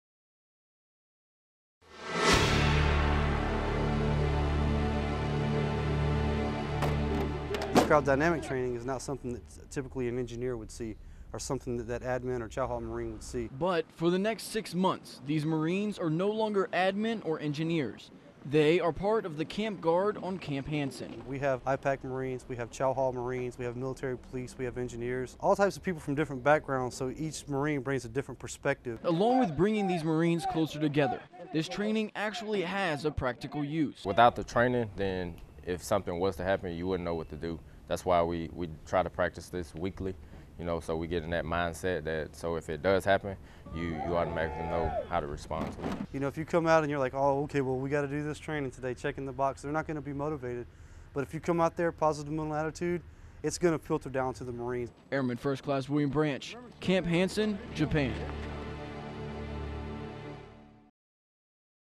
Guarding Camp Hansen
Security is key at any military base, especially overseas.  Airman 1st Class William Branch takes us to Camp Hansen, Okinawa, where a specialized unit is taking part in training that not everyone gets to experience.